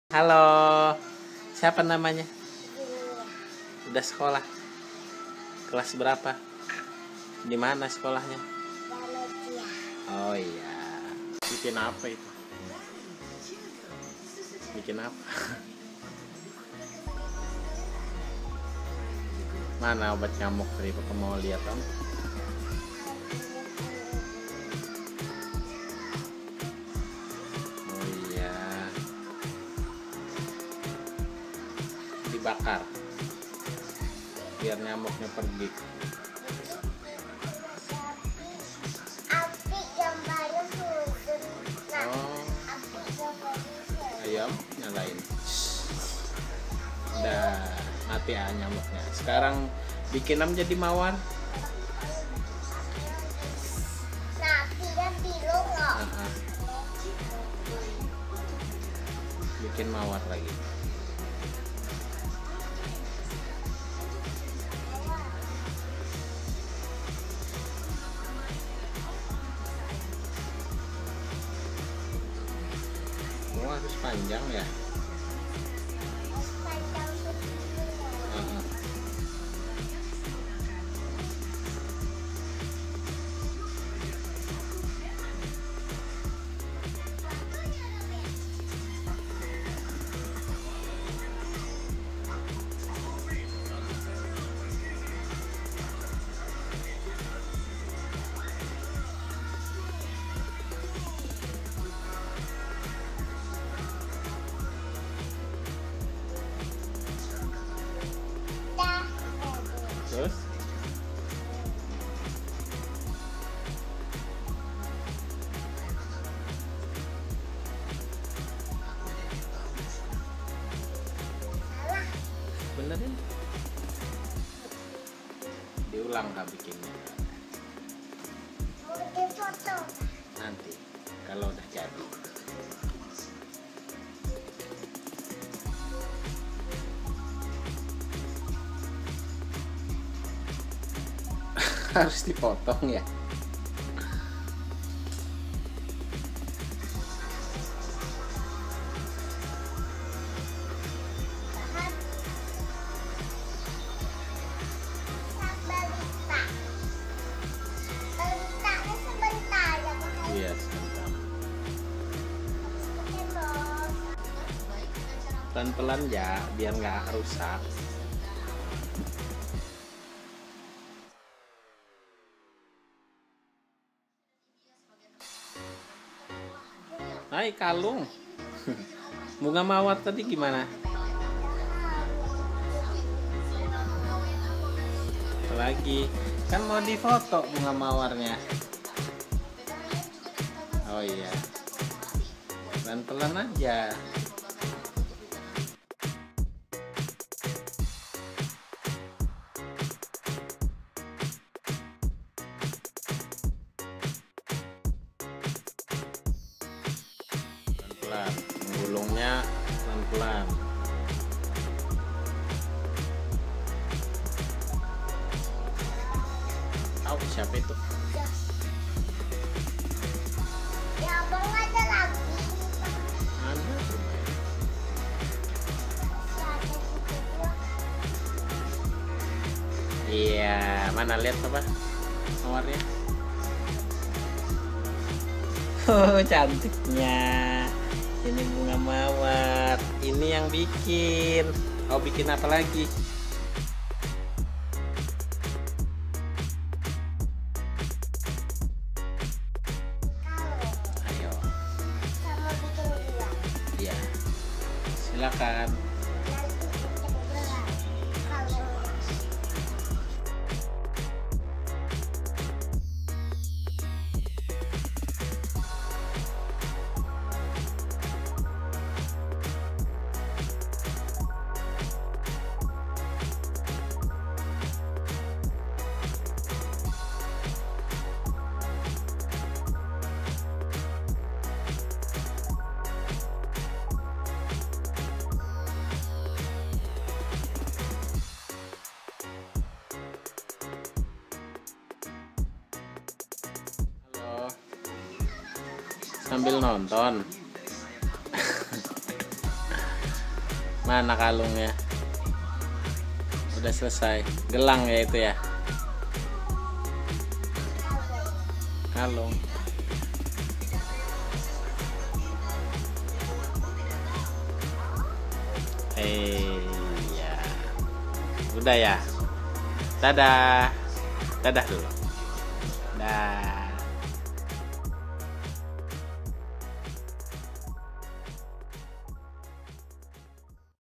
Cara memainkan Fun Doh / Play Doh tanpa cetakan untuk pemula
Zizi setahun yang lalu saat masih umur 4 tahun, bikin kreasi sendiri dari lilin mainan, mudah dipelajari dan sangat gampang ditiru untuk teman-teman yang baru mulai bermain Fun Doh / Play Doh...